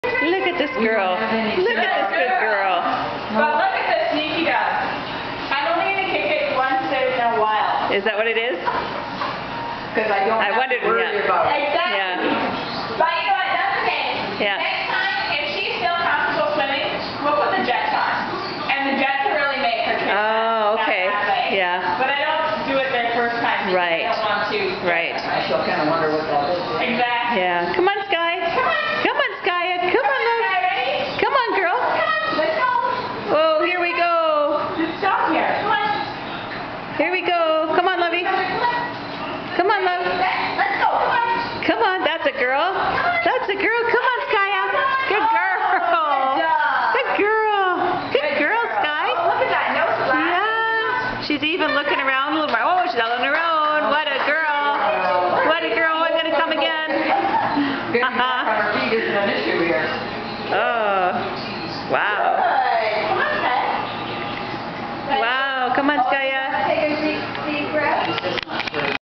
Skya Benwick & Tiffany Enjoying The Rehab Pool
Look at Skya go as she swims around the rehab pool to strenthen the recent cruciat ligament she recently had surgery on. She gives it her best and has accelerated her healing as a result!